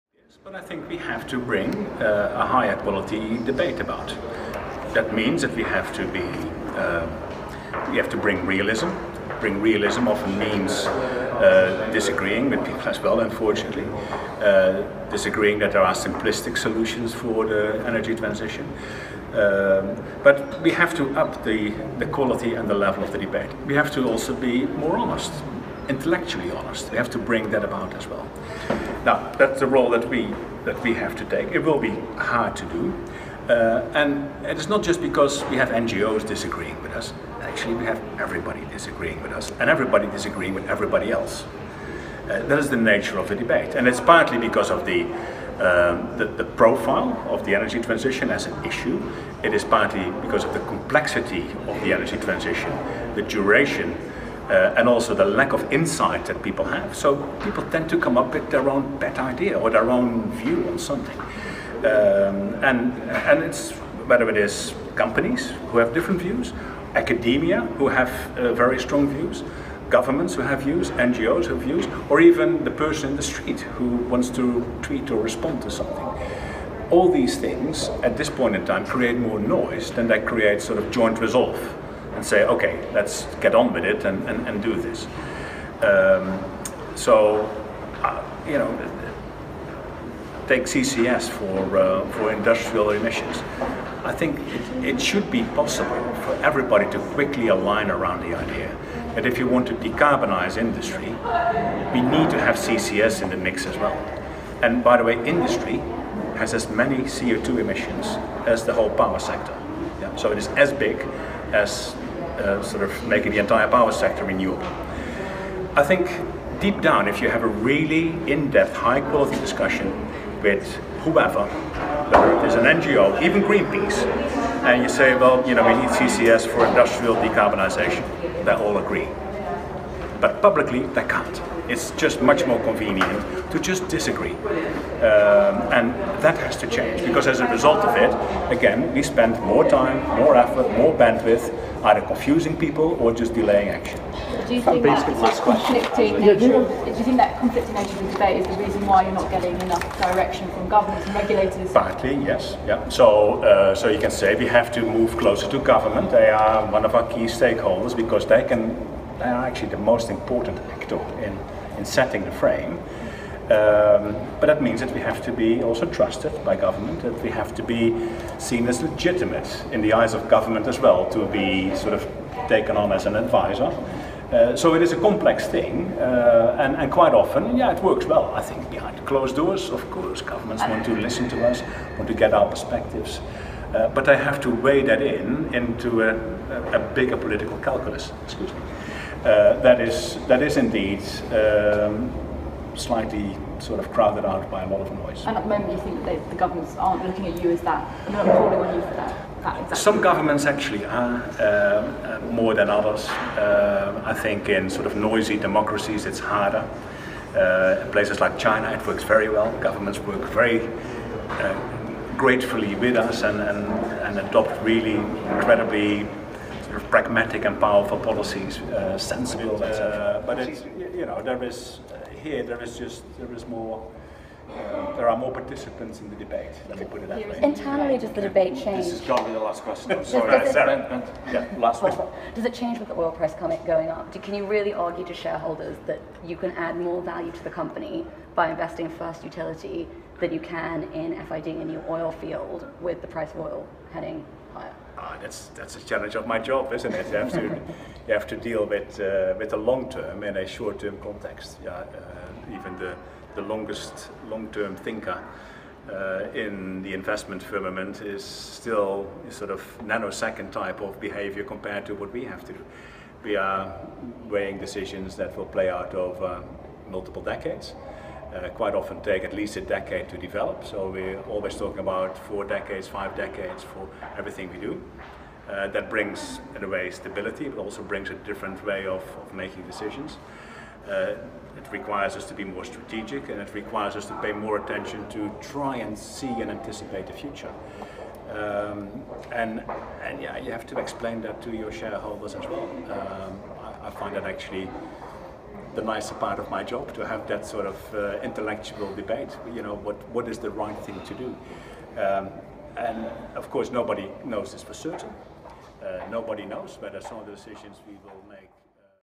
Shell CEO - London July 2018
Ben van Beurden talking about change...